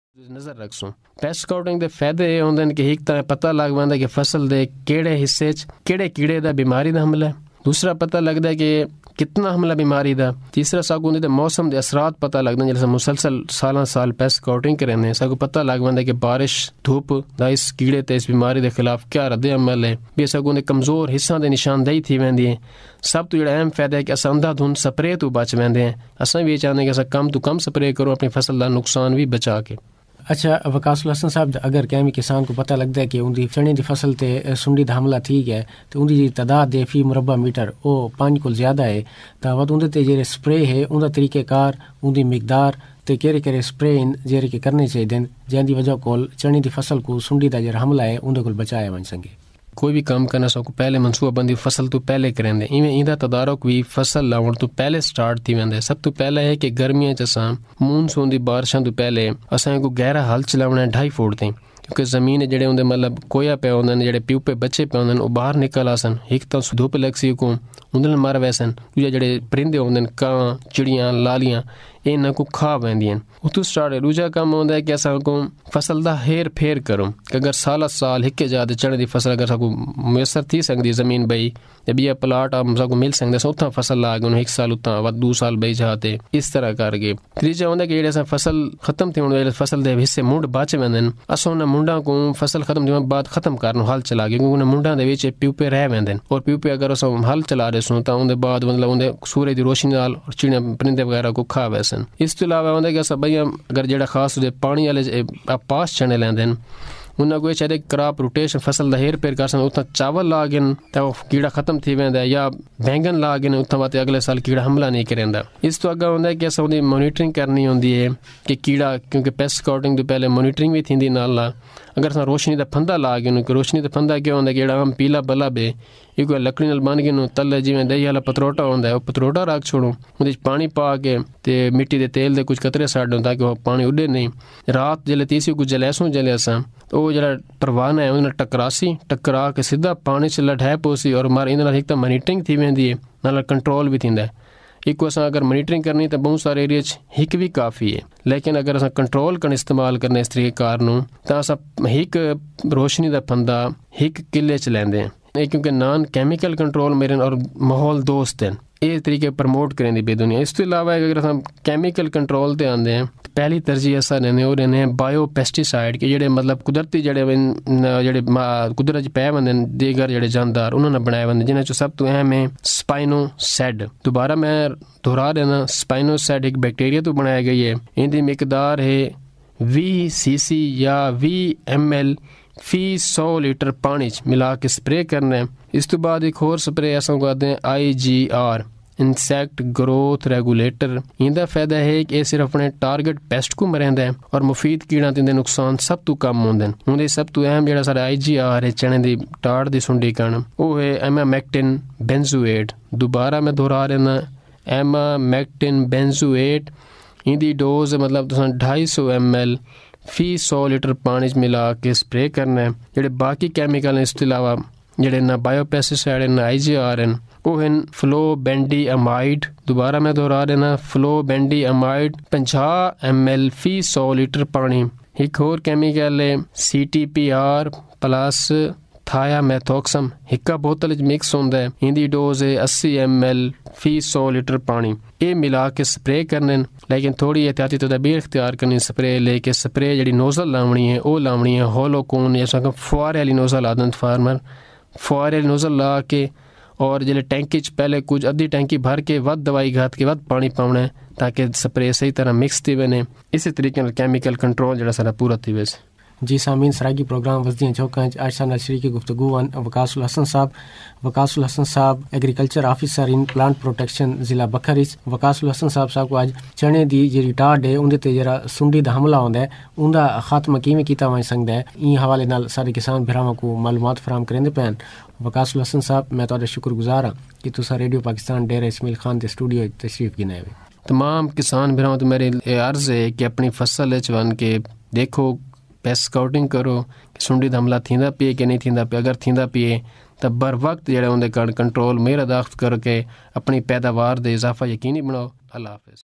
Pest Scouting, IPM of Helicoverpa ( Gram Pod Borer/Tomato Fruit Borer) ٹماٹر/چنے کی سنڈی کا تدارک
Important for Entomology/Agric Students, Field Officers and Gram & Vegetables Growers.